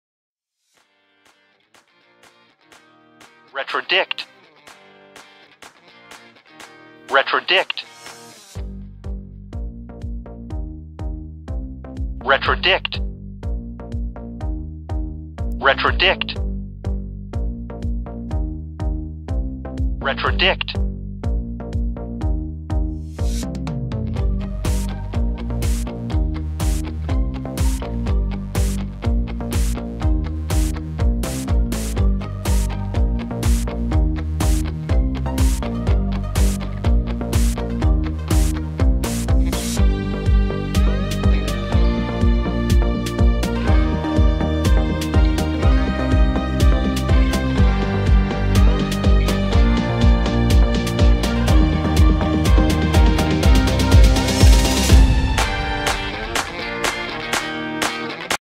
Retrodict meaning and pronunciation
Retrodict_meaning_prounciation(verb)

Meaning: to make an assumption about the past using present-day information

Sentence:The dinosaur fossils helped the scientist retrodict several different theories about what they might have eaten.
IELTS vocabulary